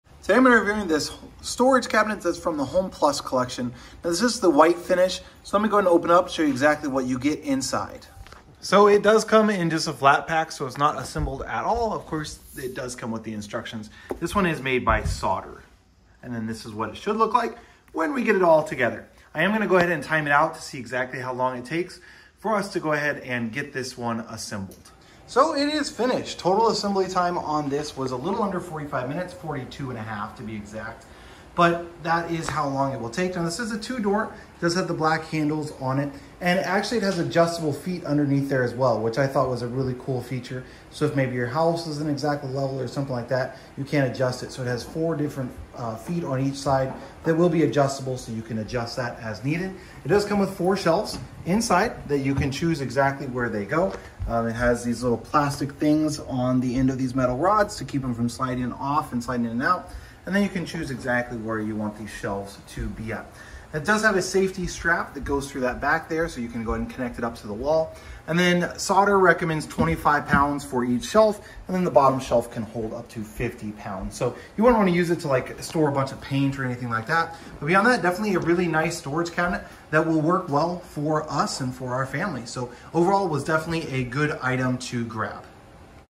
Assembly Time, Demo, & Review of my Sauder HomePlus Storage Cabinet
In today’s video, I will be reviewing my Sauder HomePlus Storage Cabinet!